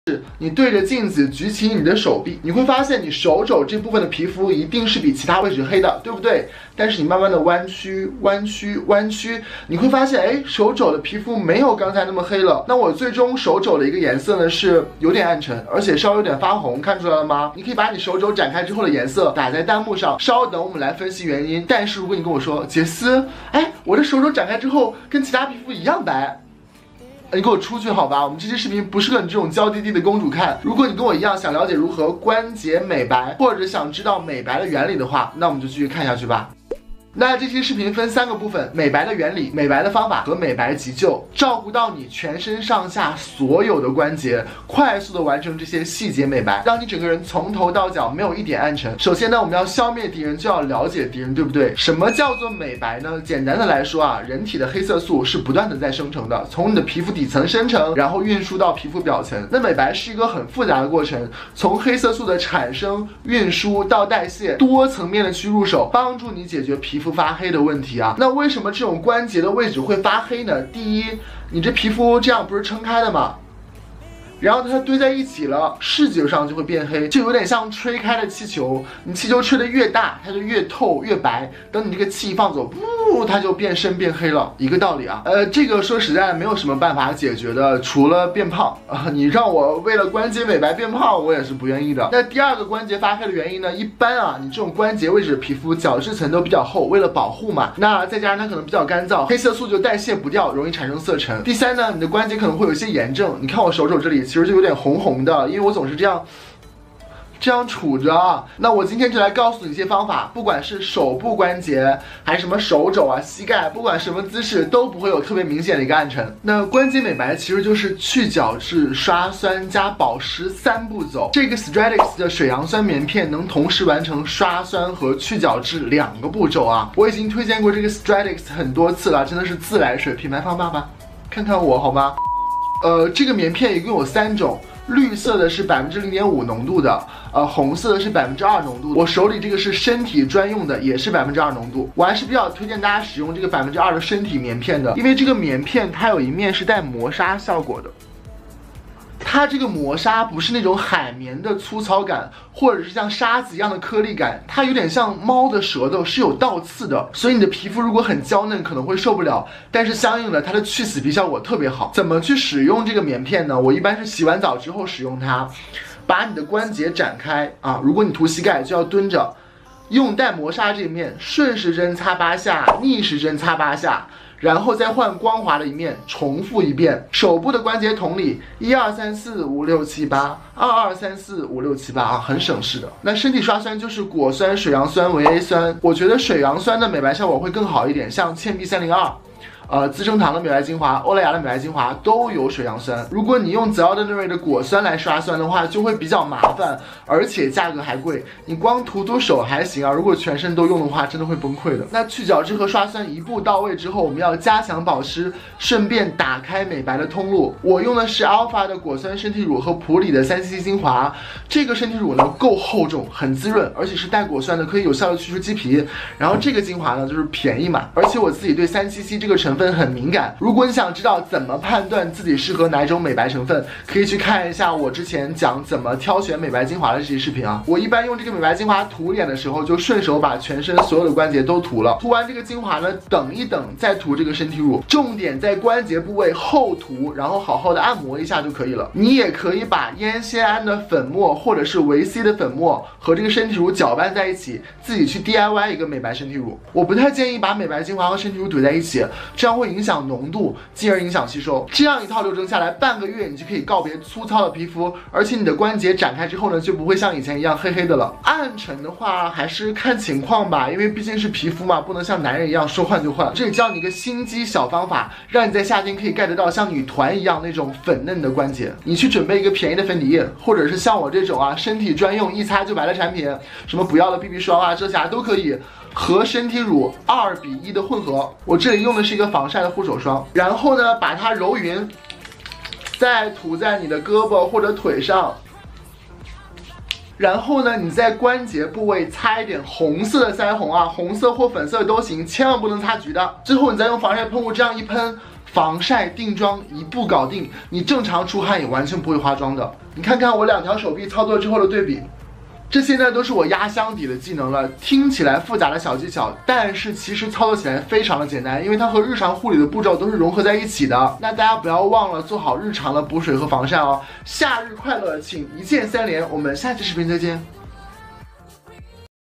超简单美白大法！3步解决关节黑，从头白到脚
本期视频
既有【美白原理关节刷酸永久白】
又有【用身体乳化妆15秒一擦白】
全身都照顾到了~
要个一键三连不过分吧